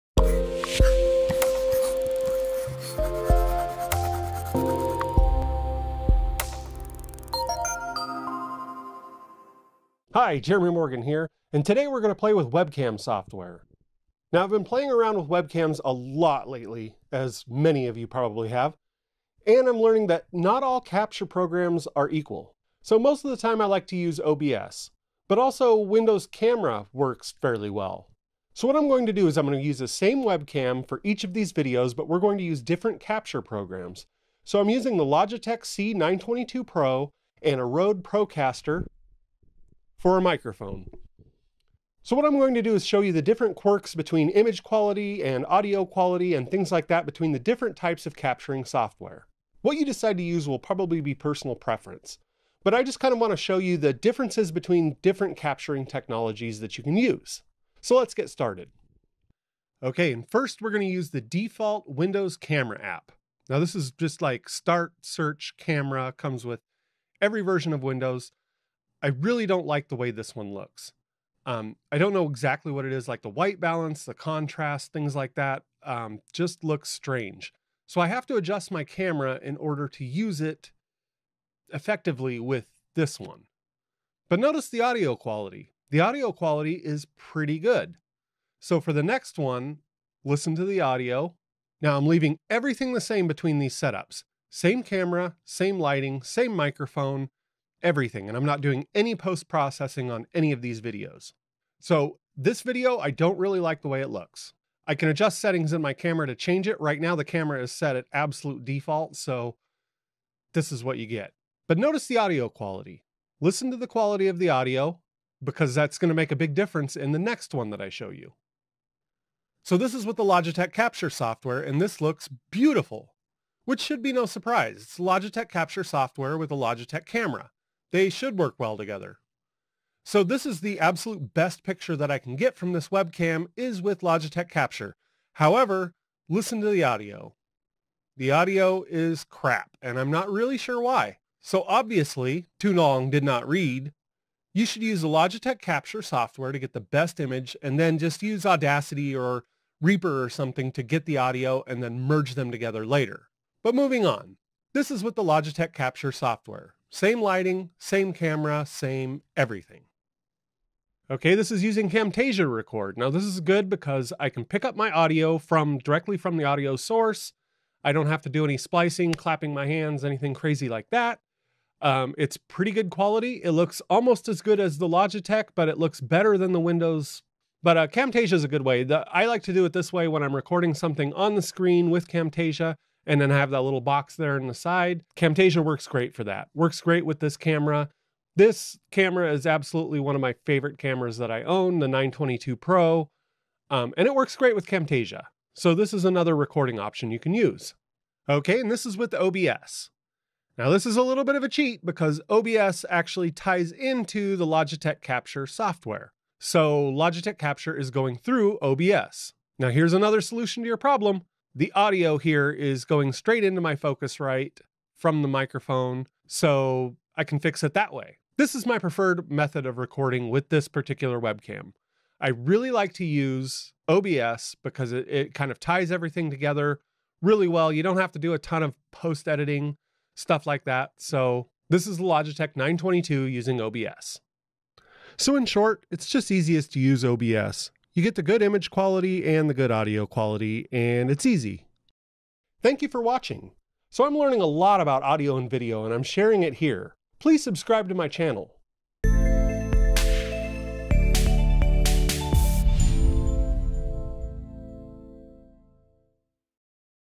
Why I use OBS to Record Webcam Video
Here I will do a comparison with the same webcam (Logitech 922 Pro), same microphone, and record it using:

Windows Camera
Logitech Capture
Camtasia
OBS Studio

And compare them all. OBS gives me the best results, as you'll see.